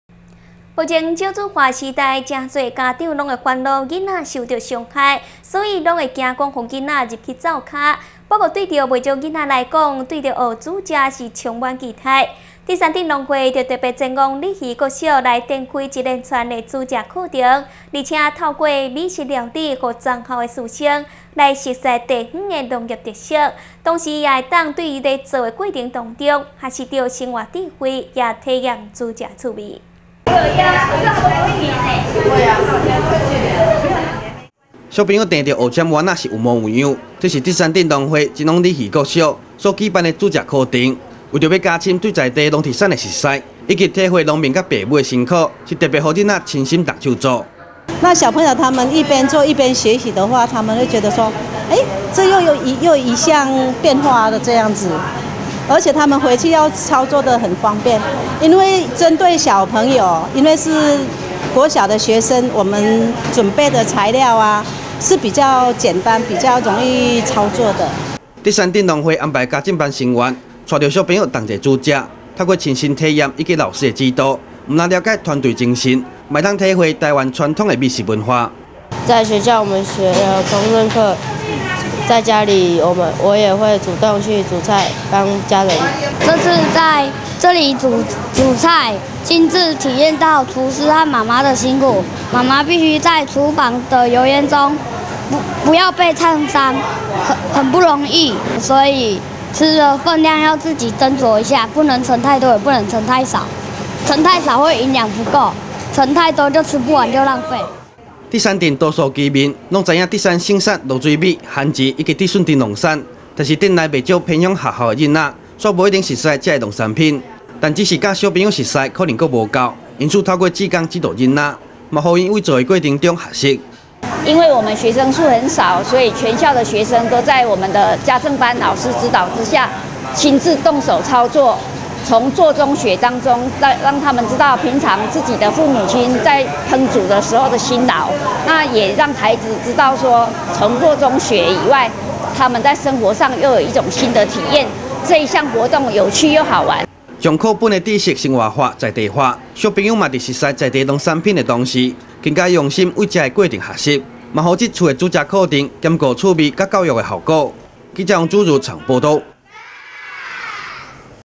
南投新聞-竹山鯉魚國小烹飪課
目前少子化的時代，多數家長會擔心，孩子受到傷害，因此都會禁止進入廚房，相對孩子，對烹飪的學習跟期待，卻是相當期望，竹山鎮農會，是特別前往鯉魚國小，展開一連串的烹飪課程，而且透過美食料理，讓全校師生，在認識地方農業特色的同時，還能從做中學習，並體驗烹飪的有趣。